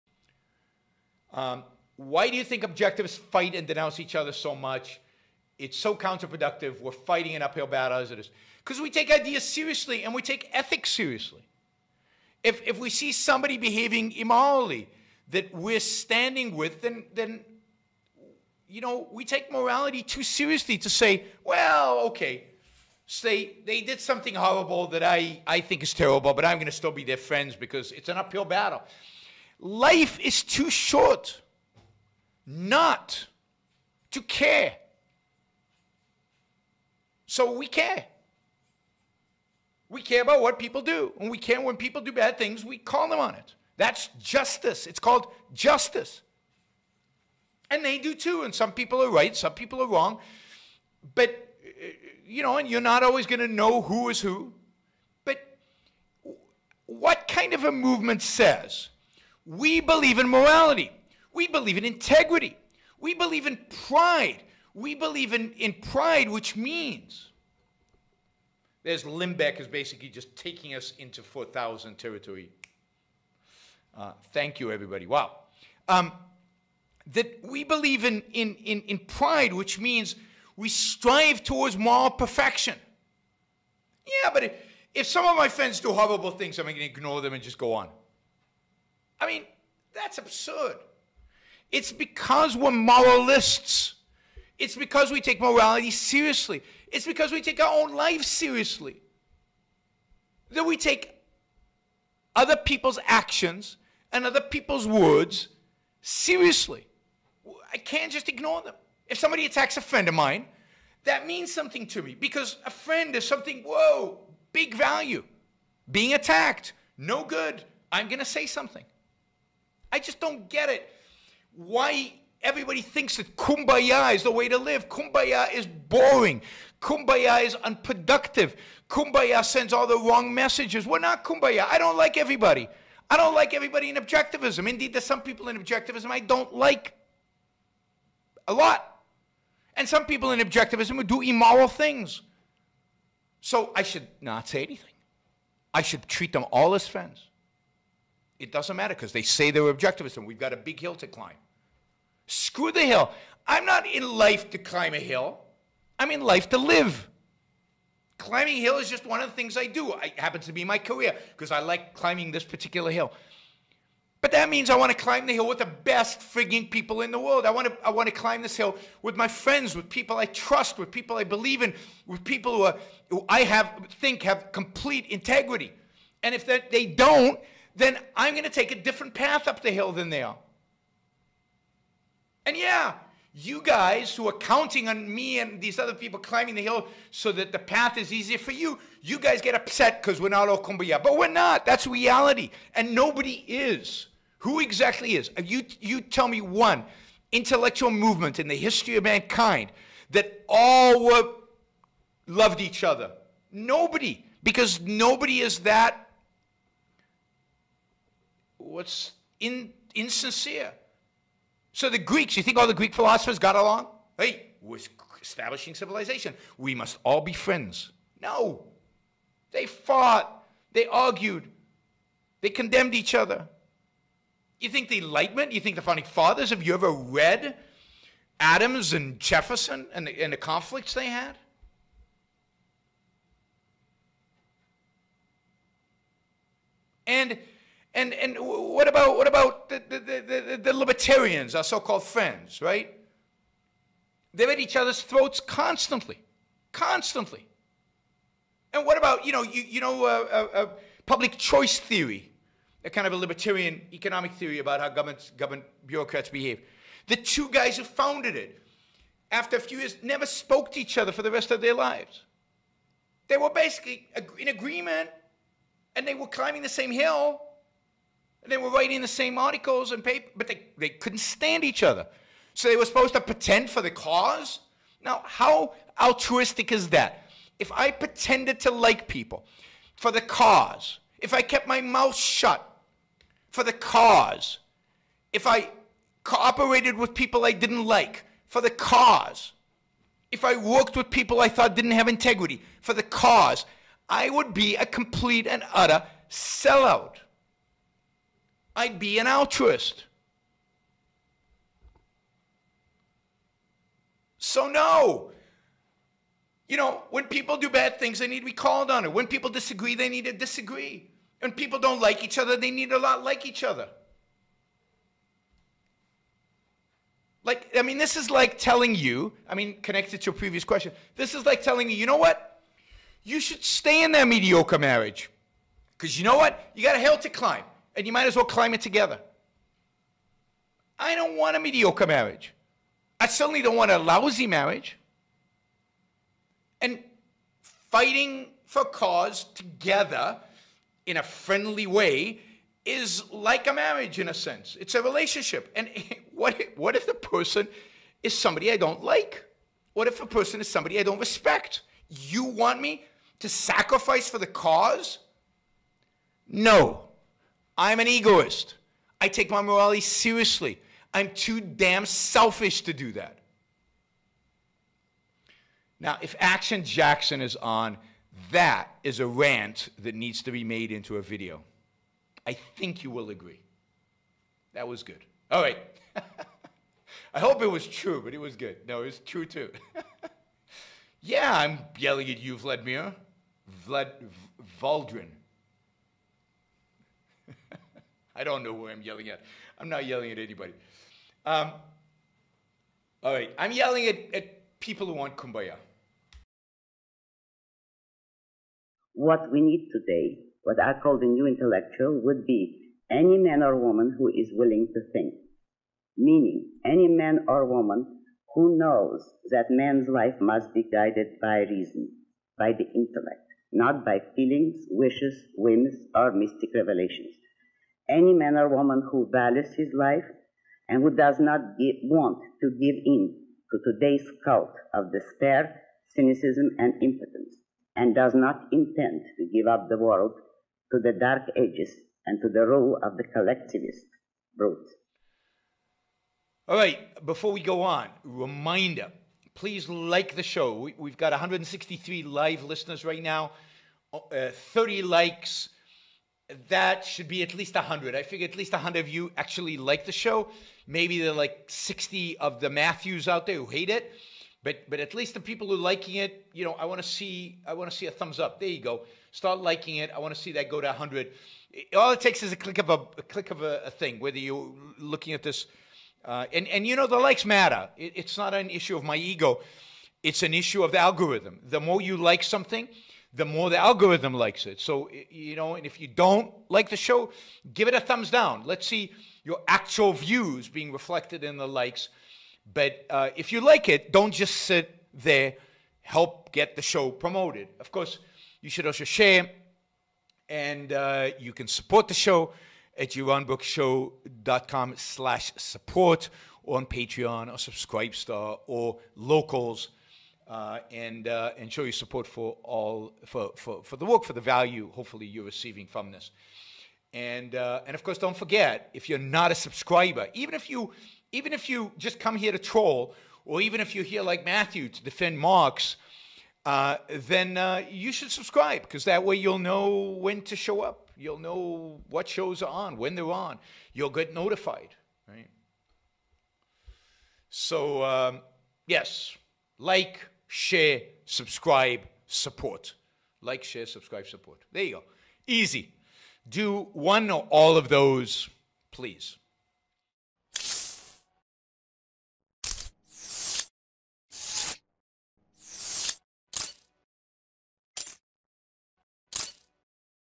I Am an Egoist, I Reject Kumbayah
This video was created by Christian Jackson.  Taken from YBS: Muhammed Cartoons, Free Speech and France; TV Show Recommendations; +++
Streamed live October 27, 2020.